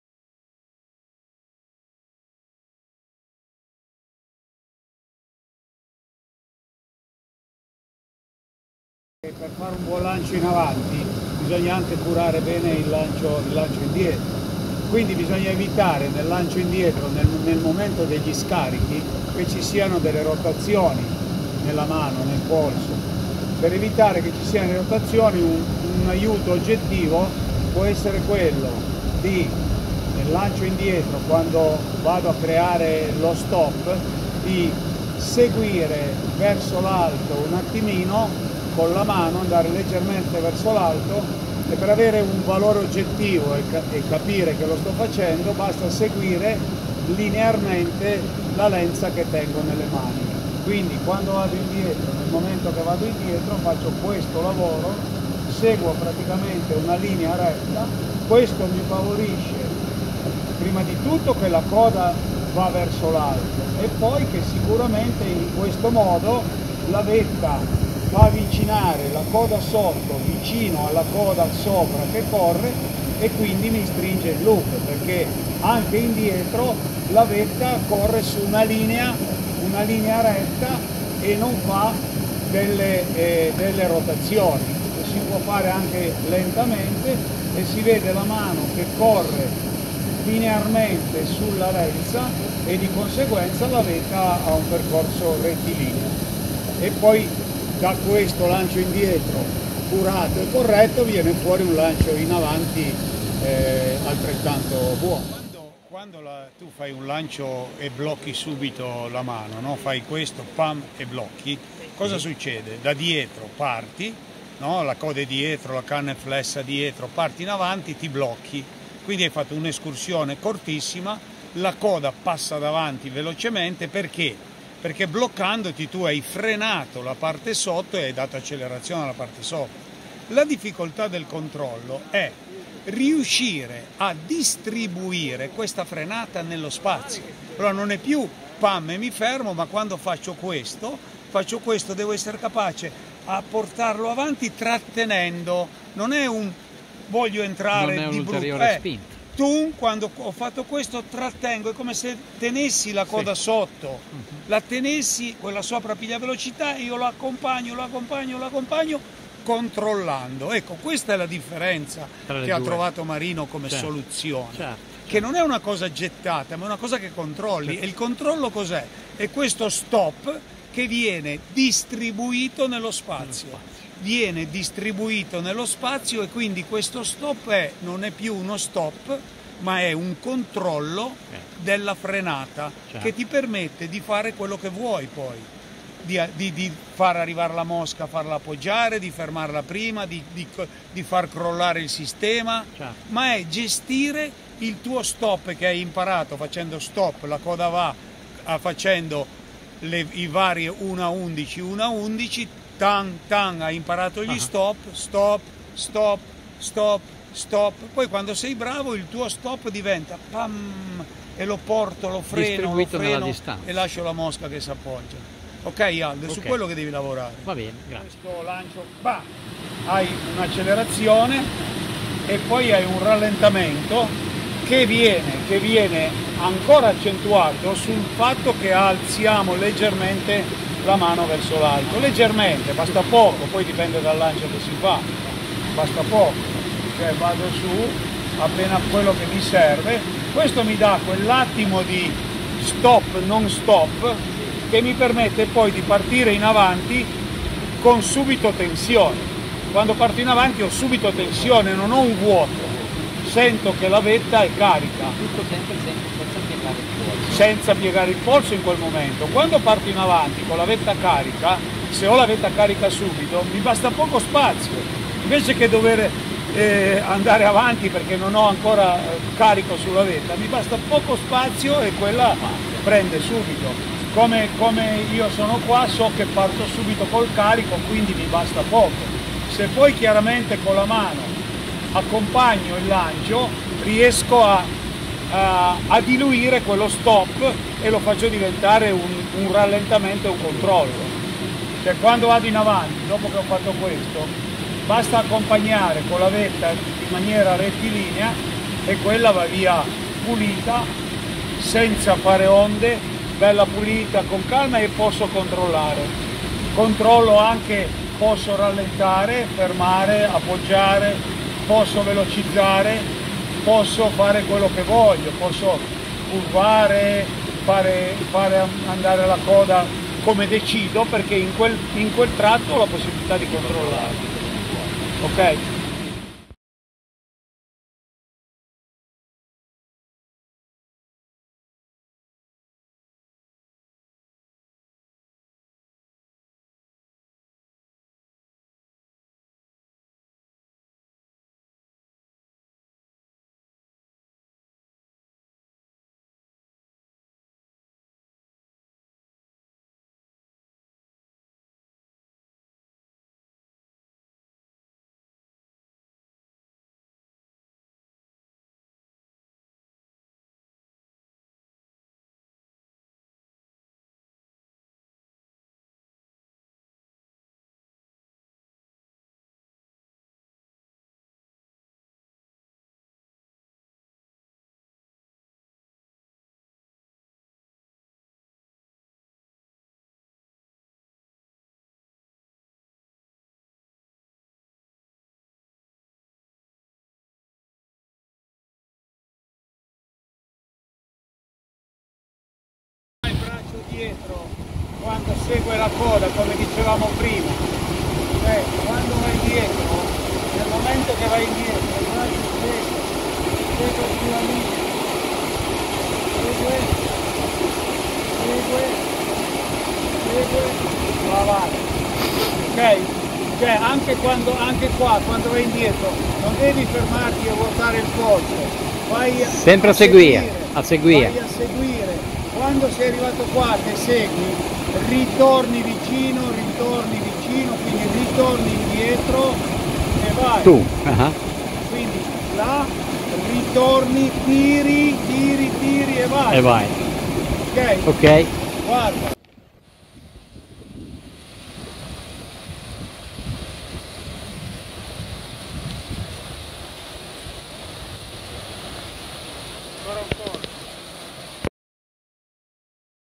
Tecnica di lancio SIM
Marco Martini. Istruttore SIM (Scuola Italiana di Pesca a Mosca) ci offre una serie di suggerimenti per migliorare la tecnica di lancio. Una corretta esecuzione ed il rispetto dei piani permette una migliore distribuzione delle spinte e una maggiore fluidità e linearità della coda.